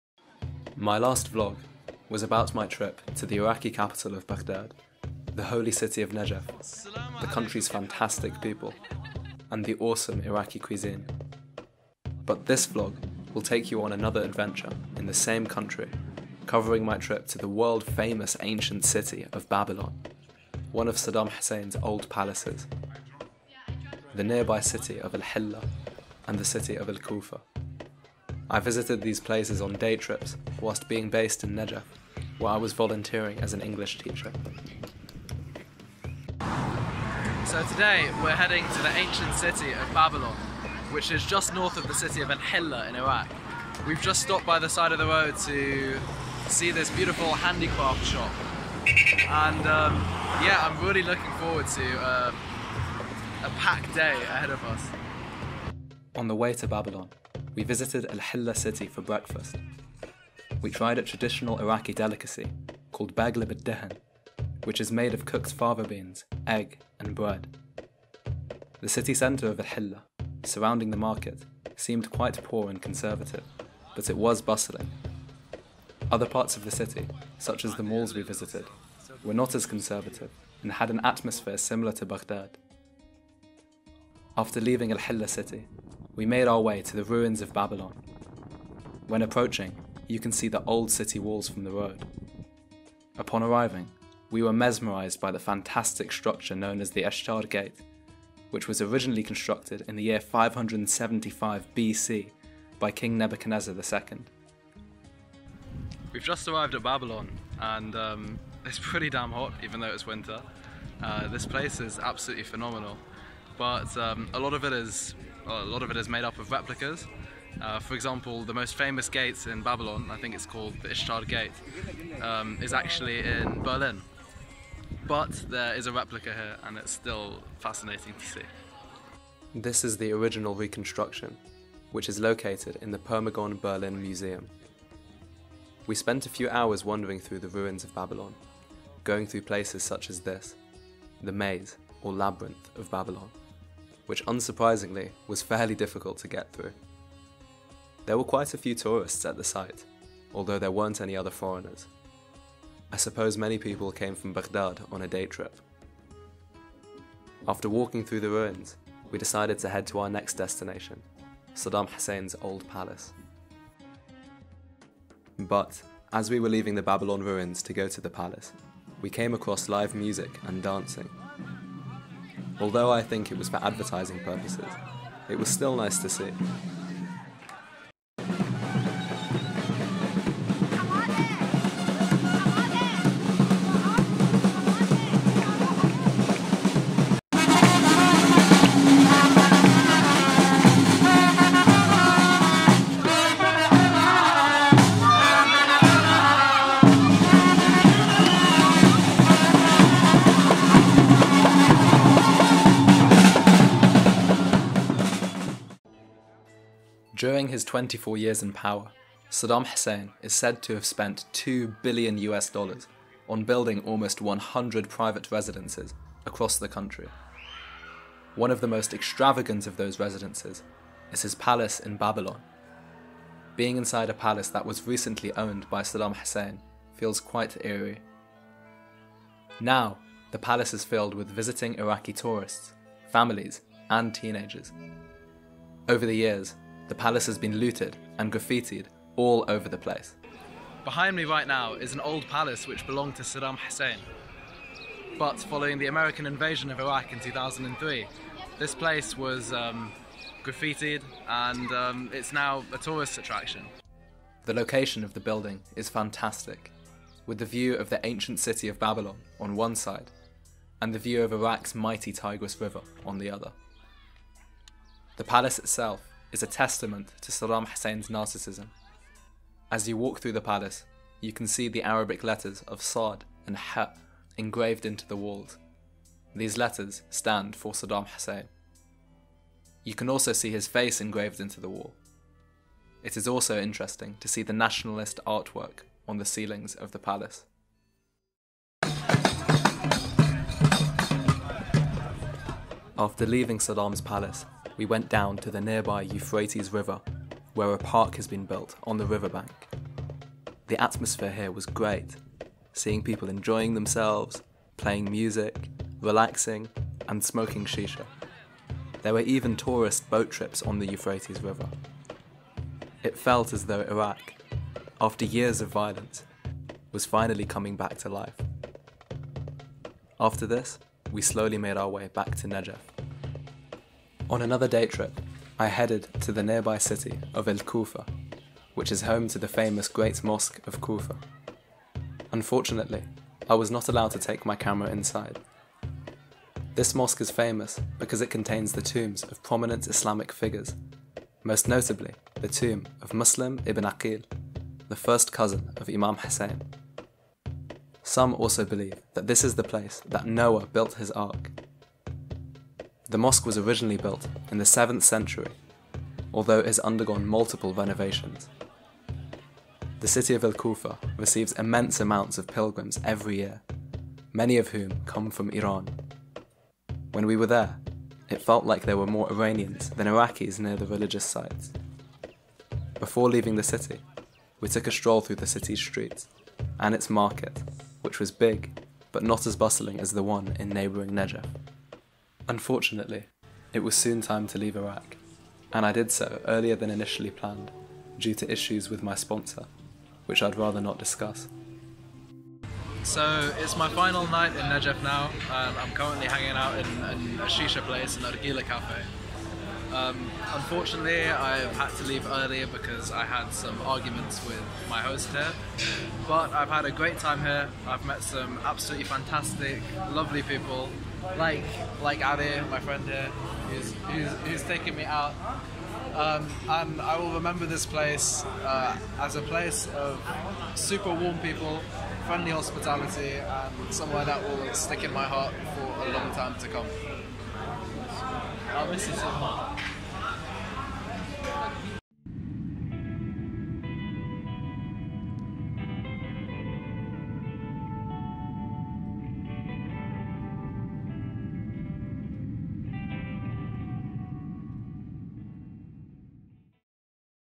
Iraq Babylon Vlog - Saddam's Palace, Al Hillah, Al Kufa بابل العراق - قصر صدام، الحلة، الكوفة
I organise group tours to Syria and Iraq, with the next one being in January 2022!

I entered Iraq as a volunteer English teacher in the city of Najaf. Throughout my stay in Iraq, I was able to travel to different parts of the country, such as Baghdad, Babylon, Al Kufa and Al Hillah. This vlog covers my visit to the ancient city of Babylon, Al Hillah city, one of Saddam Hussein's old palaces, and Al Kufah city.

Intro - 00:00
Al Hillah - 00:58
Babylon Ruins - 01:25
Saddam's Palace - 03:26
Euphrates River - 04:48
Al Kufah - 05:22
Leaving Iraq Suddenly - 06:24

This video is NOT intended to be political, which should be evident by the content of it. Please try to keep the comment section civil.